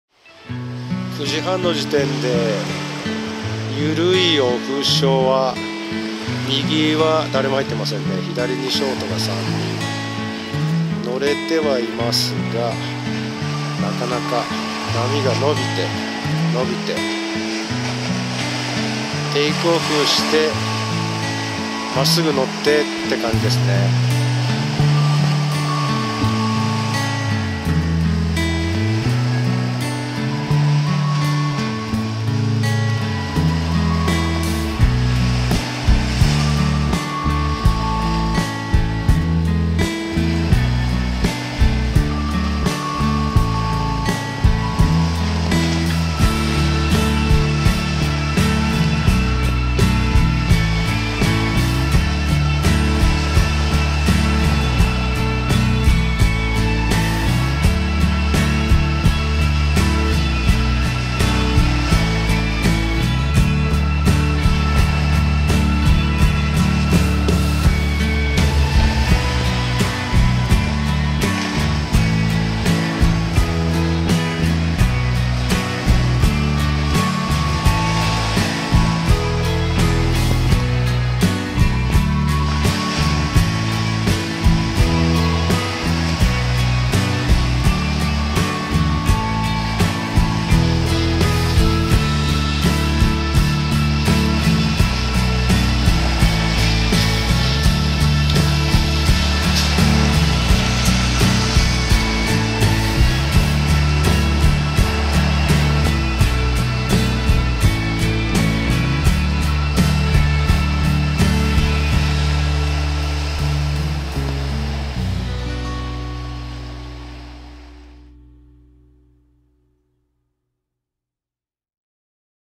つかみにくい小波にサーファたちが挑むホームポイント 200304 ~サーフモンキーTV
午前9時半頃のホームポイントは、かすかなオフショアで小波がミドル付近からブレイク。サーファーたちがなかなか掴みにくい小波に何とか乗れていました。が、自分は撮影のみで海へは入らず、別のポイントへ移動しました。よろしければどうぞ！